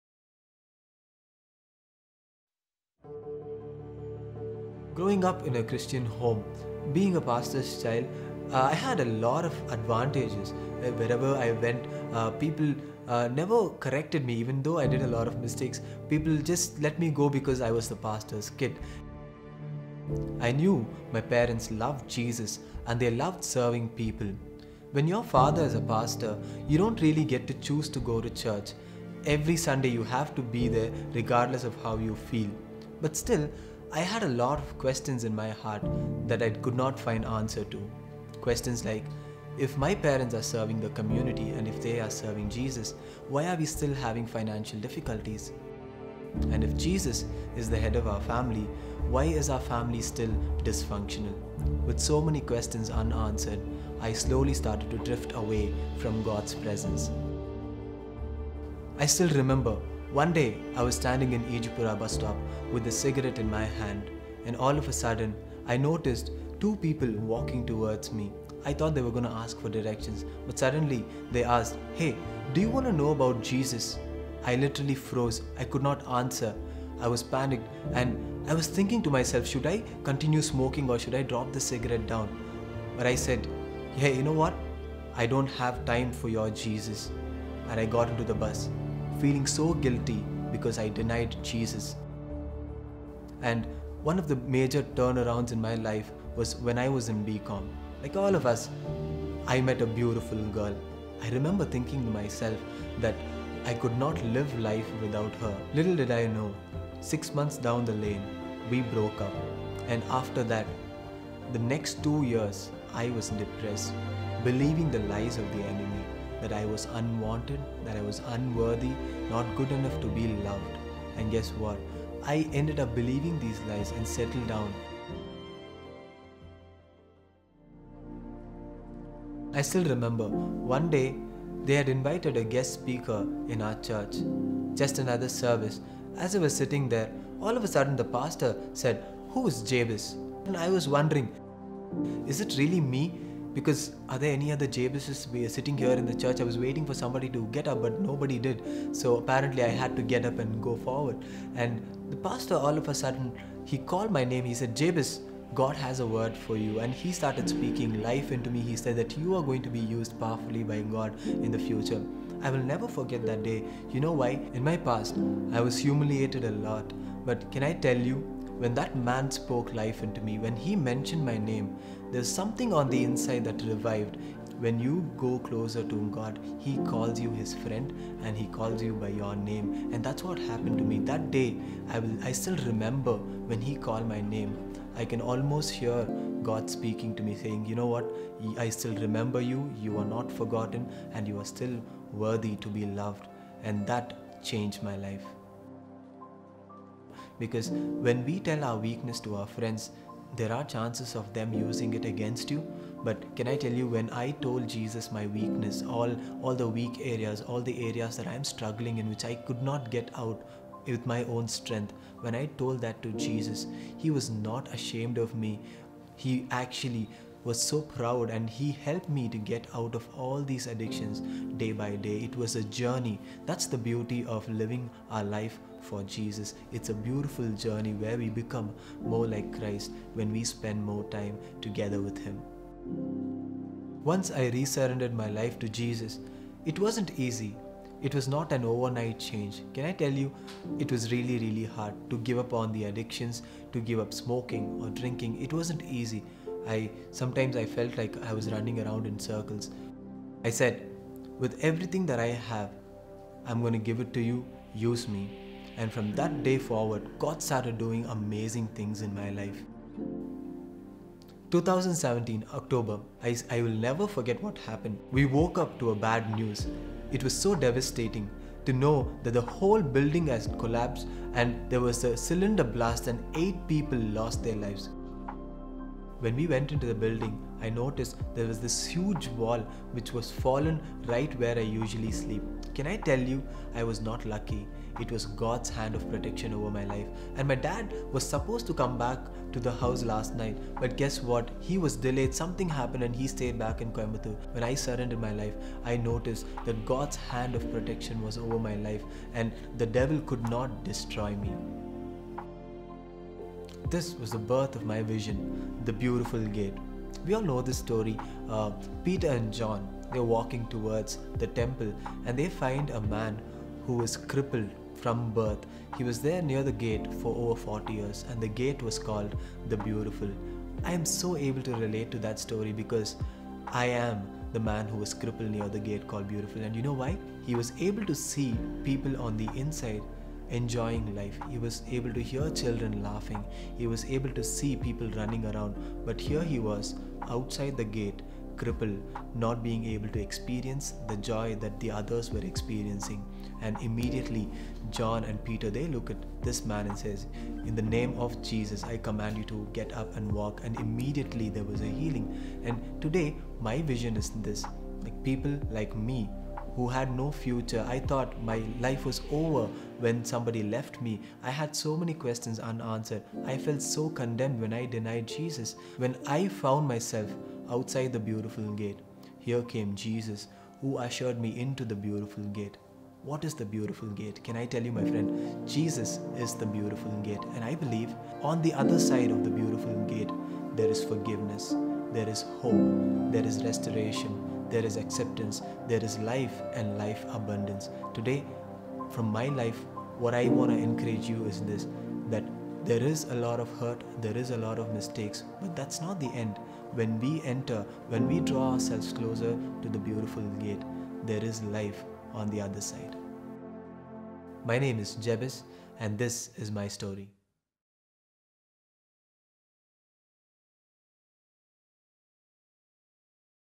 The Beautiful Gate | Jebez Johnsen (TIMS)
Jebez Johnsen - A man of his words,
Who discovered joy by following his heart into the children's ministry.
He found his true purpose by helping troubled youth identify theirs.
He lives each day, desiring to dive deeper into God's love.